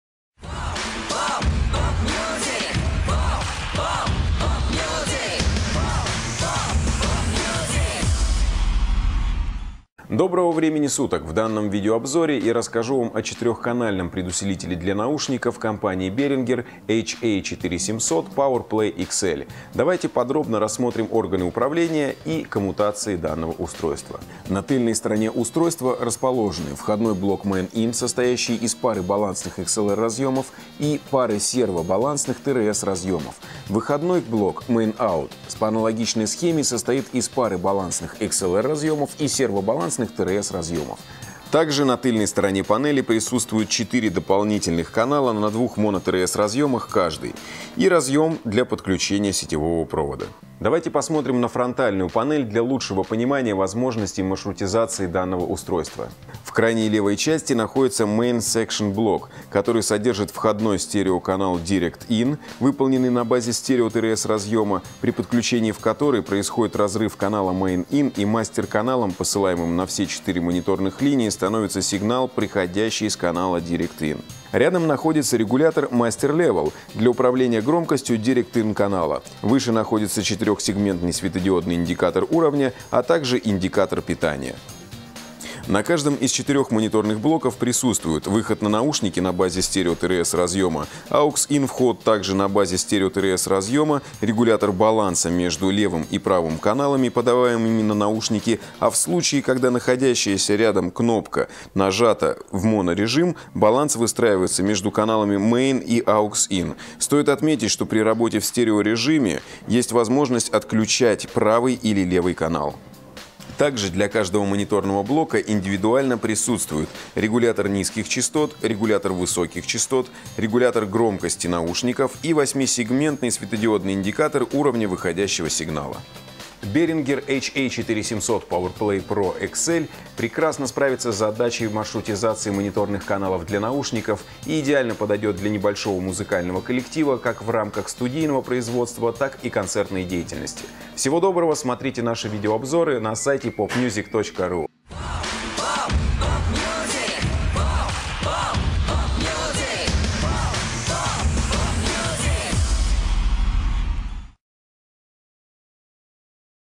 Предусилитель для наушников BEHRINGER HA 4700 POWERPLAY PRO-XL
0:10 Краткий рассказ о характеристиках предусилителя
0:24 Обзор задней панели
0:56 Обзор передней панели